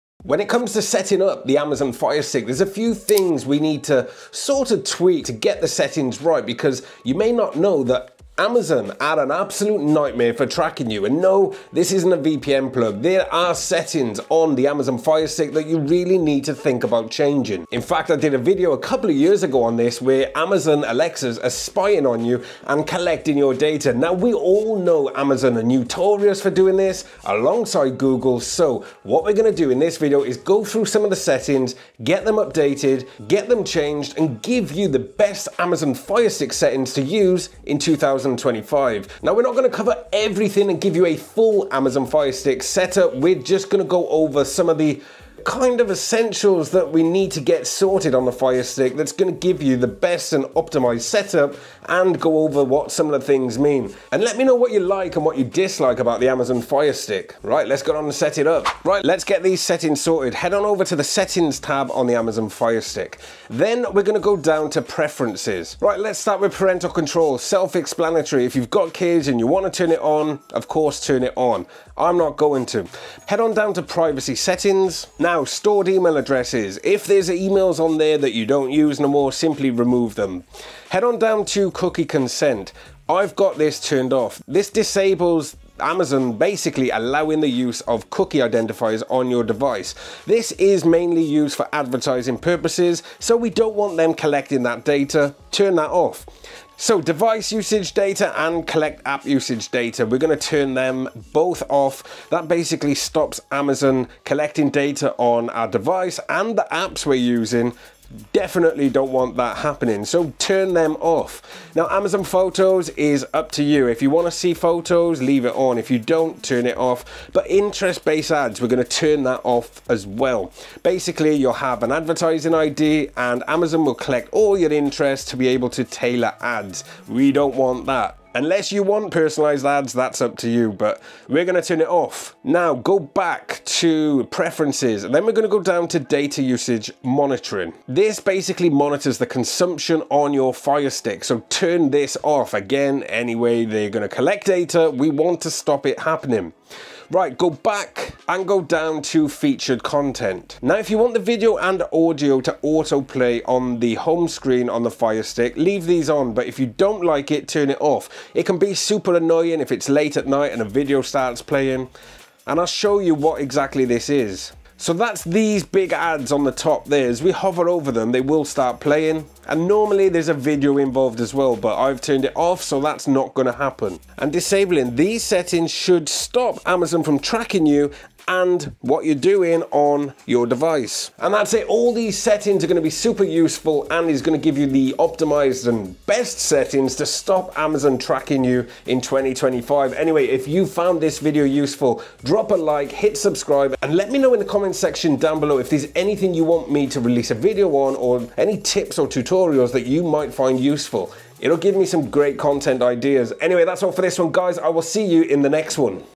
Amazon Firestick Settings you NEED to change NOW! (2025)
Amazon Firestick owners should TURN OFF these settings NOW! These Firestick settings are COLLECTING your data!

These Amazon Firestick settings are often missed in most other videos, you REALLY need to change these settings first when setting up the Amazon Firestick!

In this video, we take a DEEP DIVE into the advanced Amazon Firestick Settings to use this year.

Regardless if you’re a beginner or advances user of the Fire TV, this is something everyone NEEDS to do!

Credits to David Cutter for Music I use in my videos